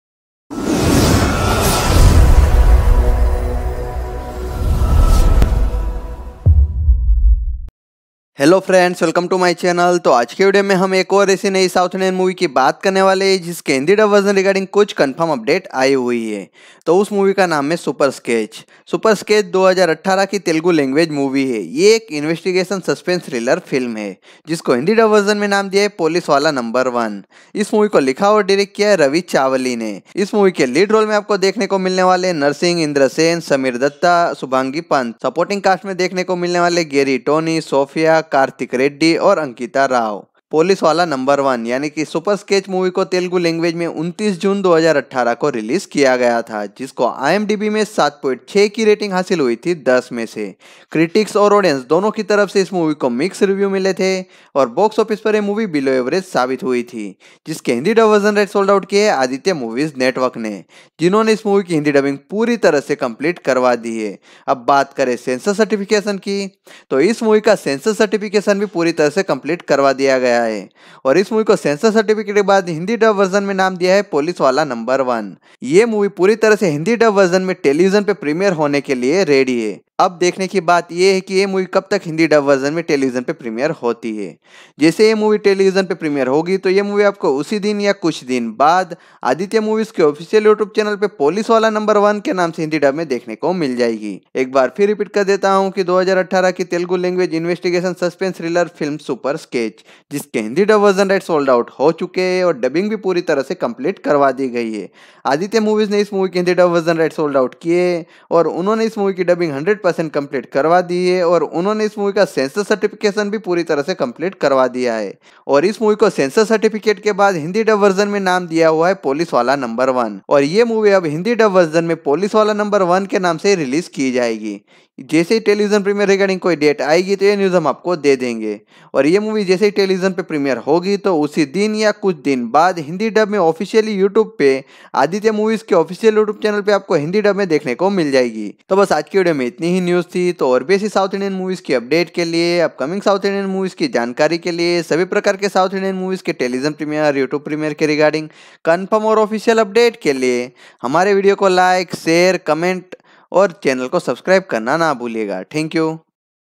Super Sketch Full Movie Hindi Dubbed 2020 | Policewala No. 1 South Movie In Hindi | Confirm Update
Checkout Full Video To Get Full Hindi Dubbed Confirm Update About Telugu Language Movie Super Sketch ( Policewala No. 1 ).

Star Cast : Narsing, Indrasena, Sameer Datta, Shubhangi Pant